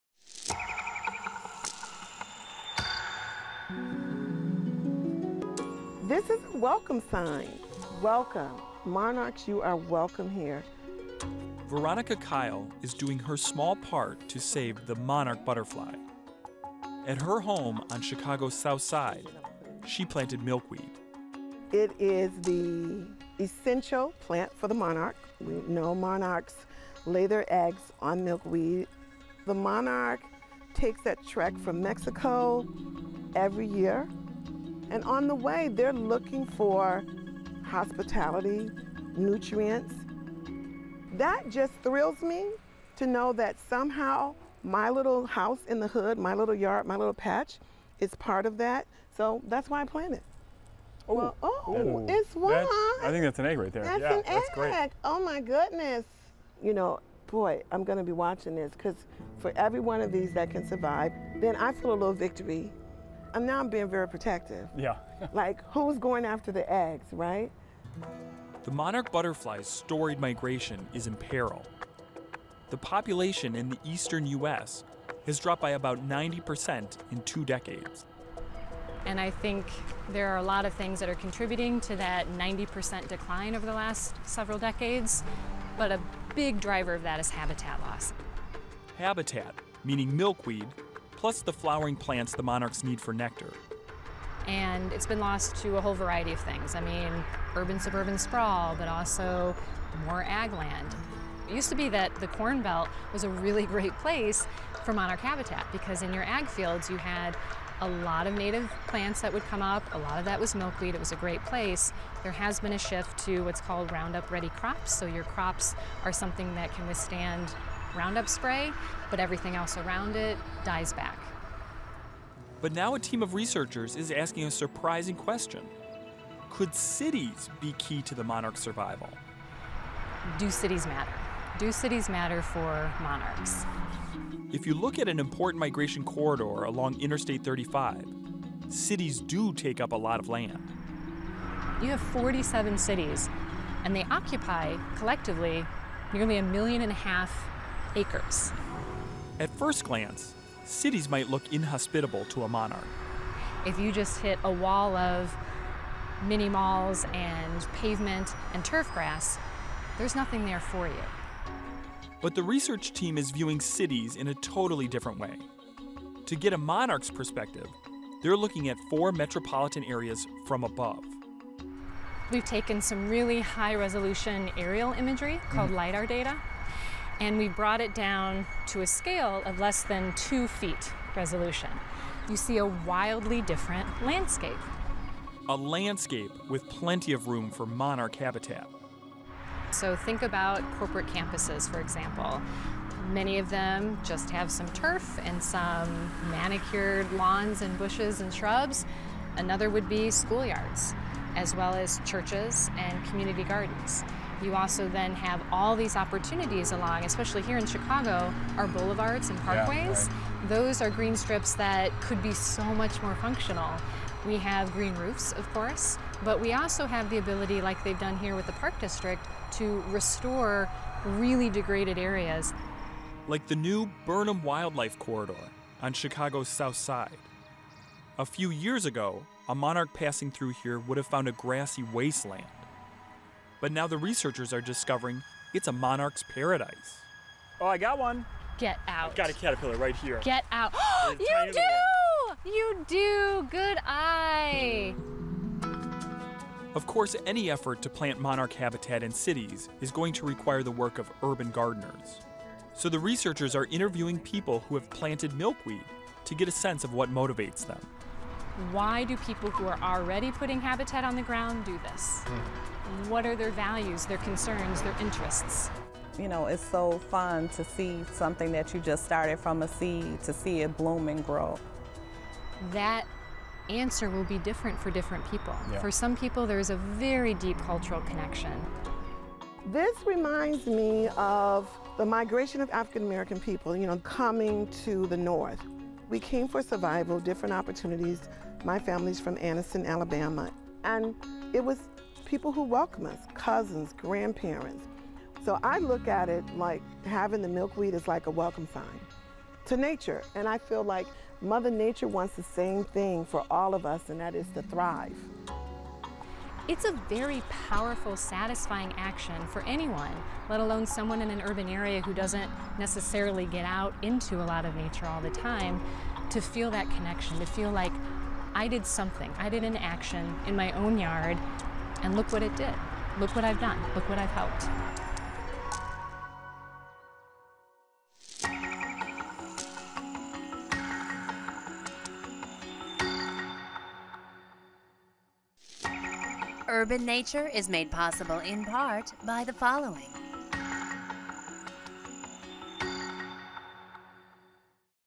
Can Cities Save the Monarch? — Urban Nature
The monarch butterfly’s remarkable migration is in peril. Its habitat has been decimated by rapid urbanization and changing agricultural practices. Could cities come to the rescue?